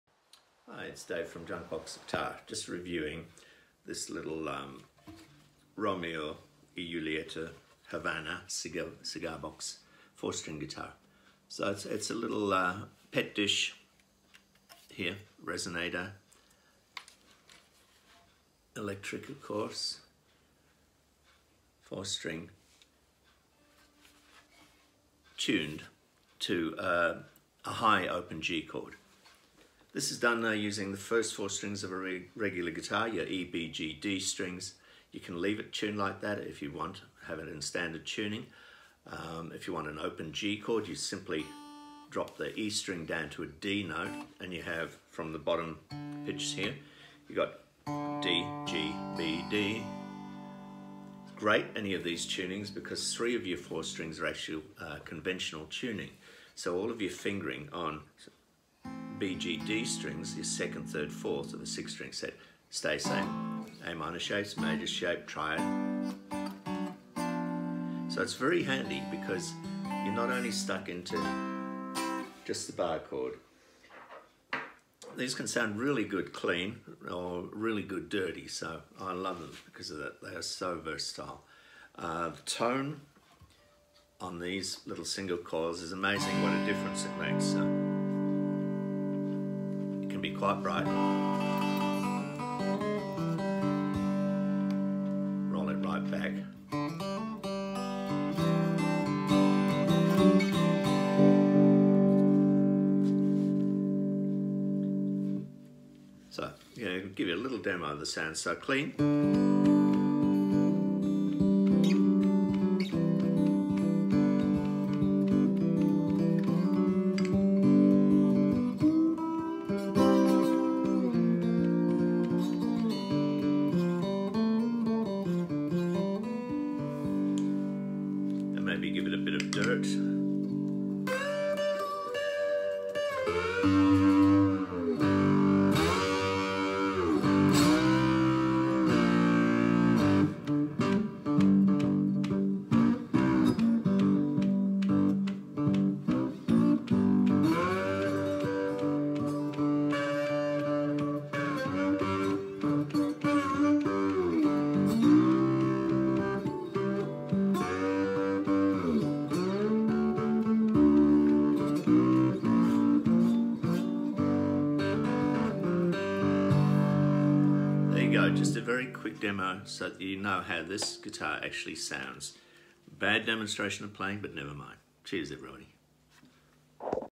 4 string cigar box guitar, Romeo y Julieta box, High Open G: DGBD tuning
A brief demo of a cigarbox guitar tuned to high open G